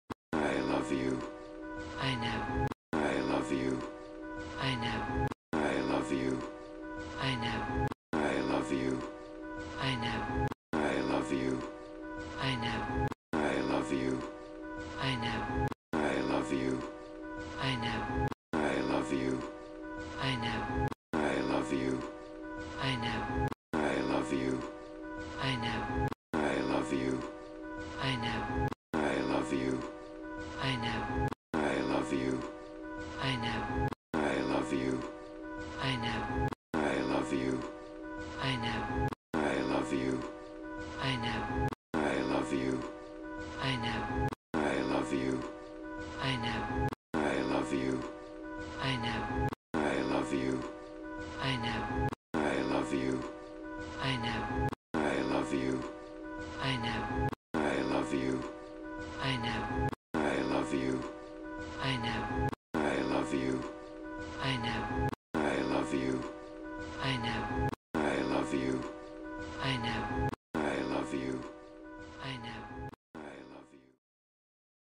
Han and Leia (both of my old parents)
Han and Leia are both of my old parents and married for 29 years.